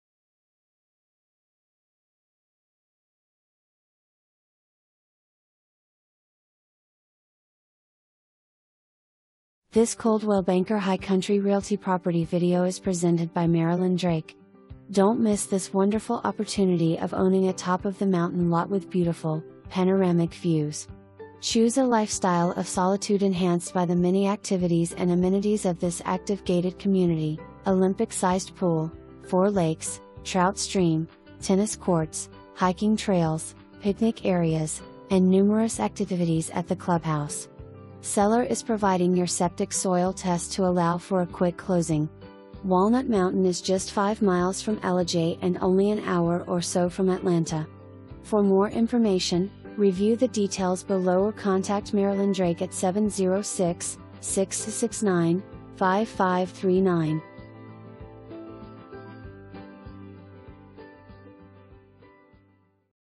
Coldwell Banker High Country Realty - WALNUT RIDGE-LT L67
Don’t miss this wonderful opportunity of owning a top-of-the-mountain lot with beautiful, panoramic views! Choose a lifestyle of solitude enhanced by the many activities and amenities of this active gated community: Olympic sized pool, four lakes, trout stream, tennis courts, hiking trails, picnic areas, and numerous activities at the clubhouse. Seller is providing your septic soil test to allow for a quick closing. Walnut Mtn. is just 5 miles from Ellijay and only an hour or so from Atlanta.

Marilyn Drake
Coldwell Banker High Country Realty